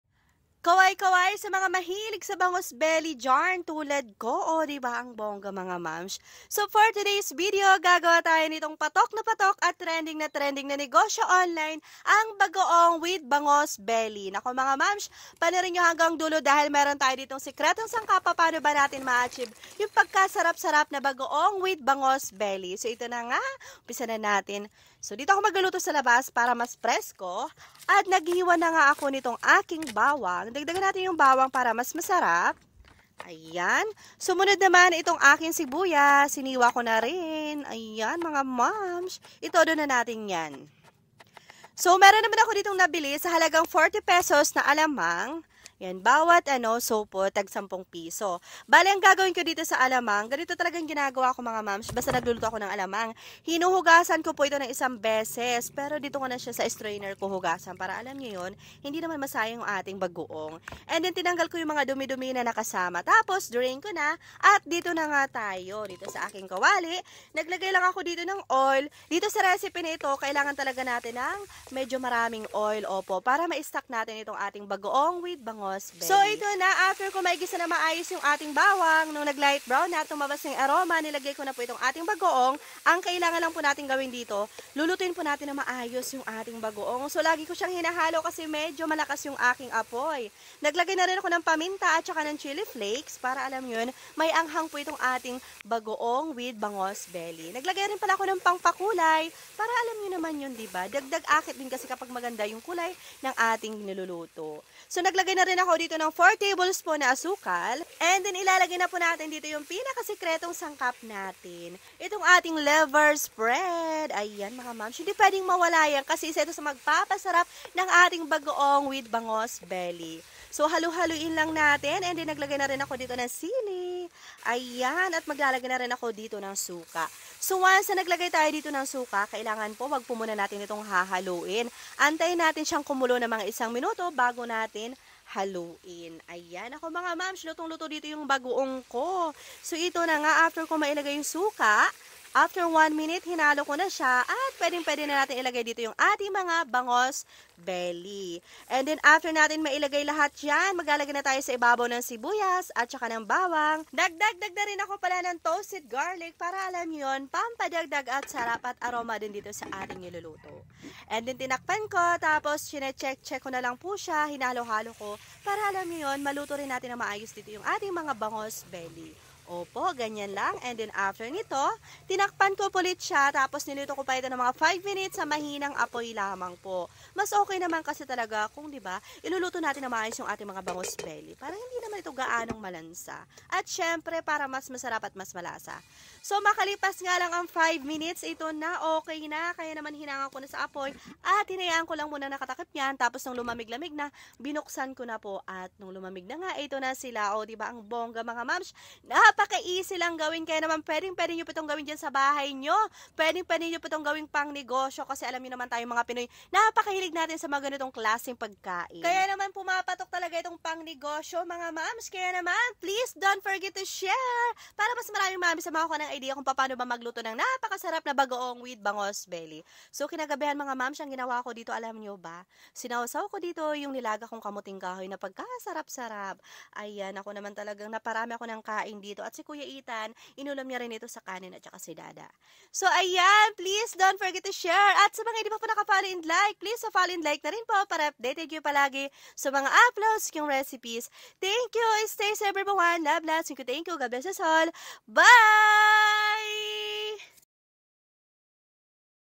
Bagoong with Bangus Belly Recipe/ Patok Pang Negosyo/ Bagoong Recipe/Bangus Recipe
Ingredients
Bagoong
Bangus belly
Pepper
Chili flakes
Sugar
Vinegar
Garlic
Onion
Liver spread
Oil
Sili labuyo